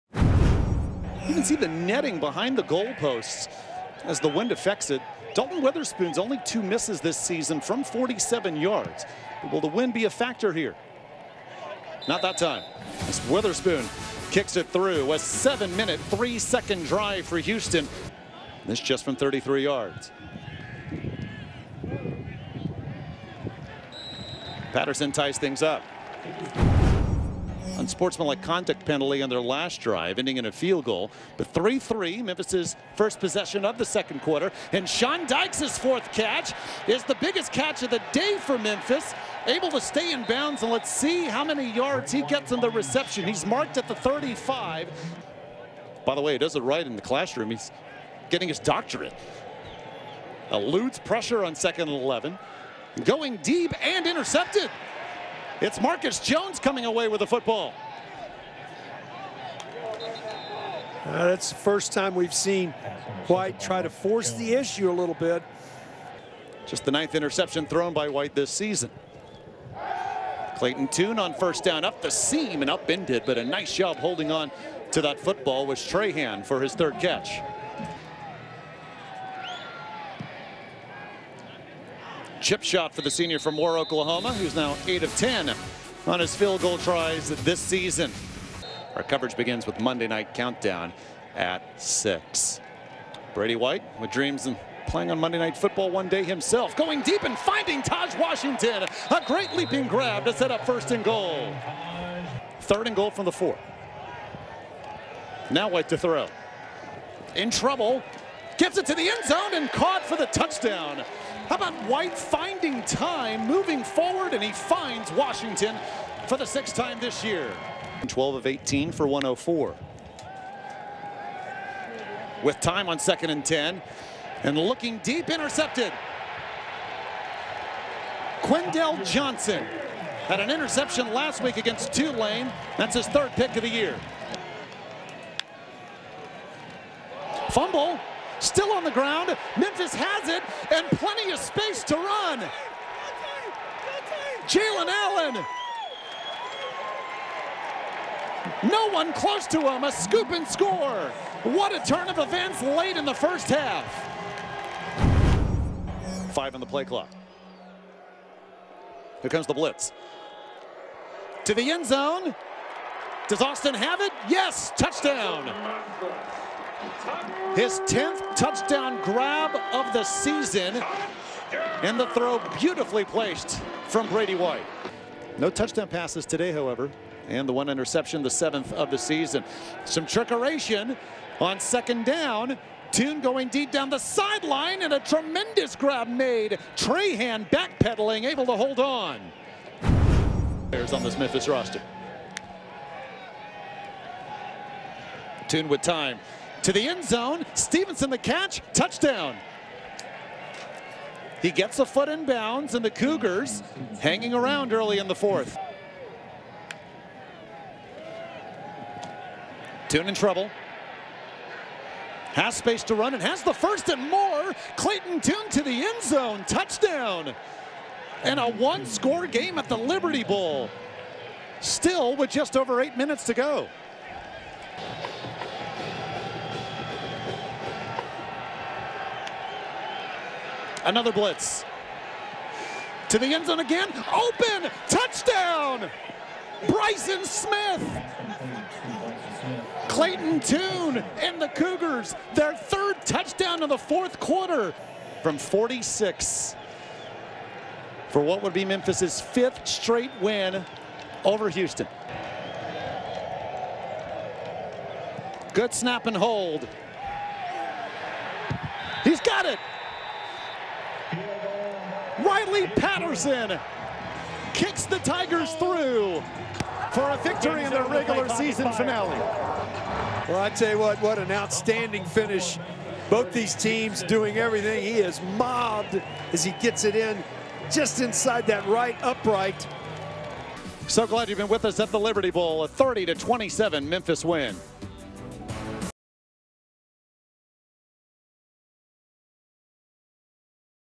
2020 American Football Highlights: Memphis 30, Houston 27
Memphis pulled out a 30-27 win over the Cougars as Riley Patterson hit the game winning field goal as time expired.

QB Brady White lead the way for the Tigers with 246 yards through the air and two touchdowns.